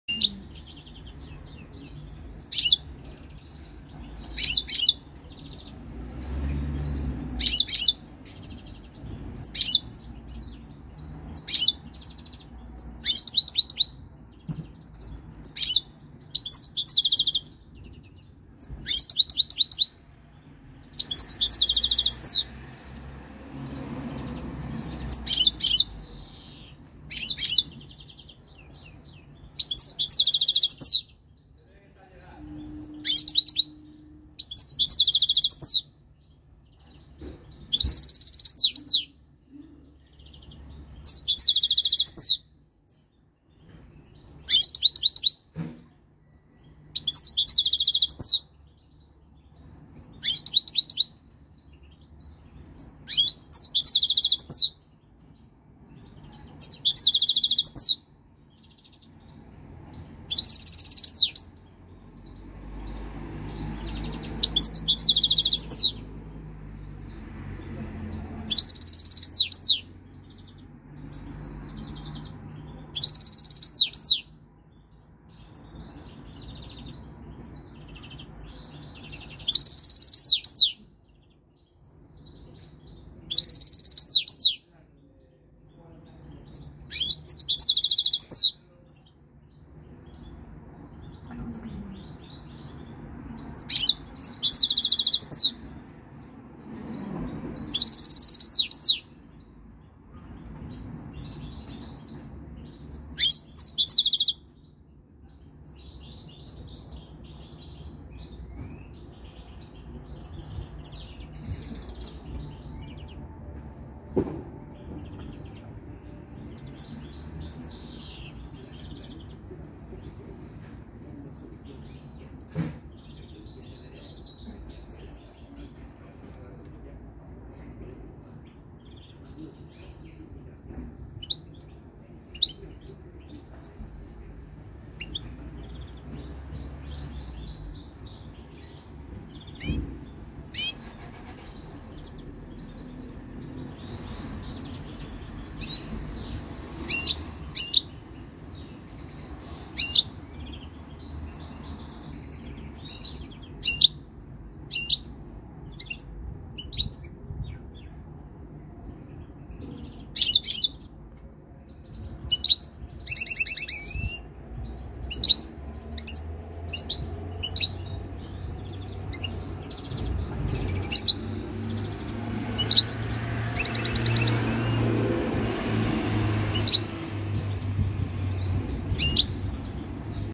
Estimulante Jilguero
Ejemplar de 3 años empezando a cojer celo, para mas videos estimulantes y consejos subscribete.